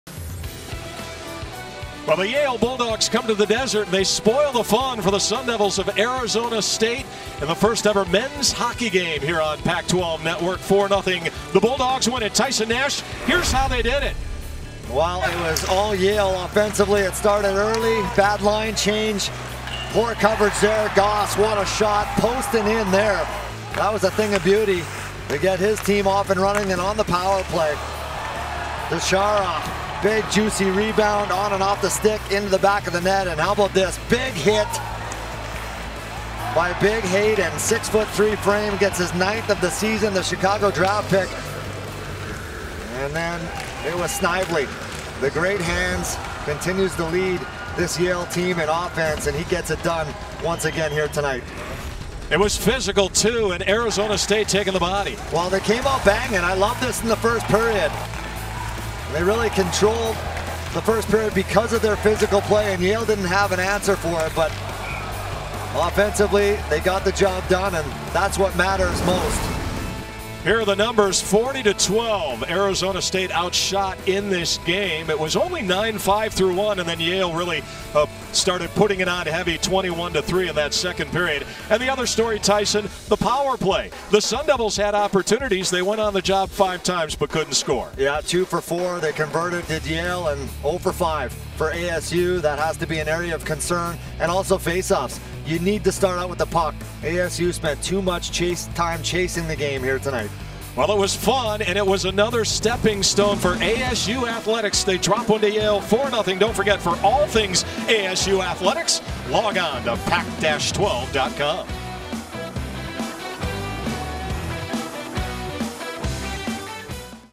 Recap: Arizona State falls to Yale in men's ice hockey
(Originally published on 01/09/2016) Pac-12 Networks' Matt McConnell and Tyson Nash discuss Yale's 4-0 defeat of Arizona State in men's ice hockey. The Sun Devils set the tone with physical play in the first period, but ulitmatley was no match for the Bulldogs' offense Friday in Glendale.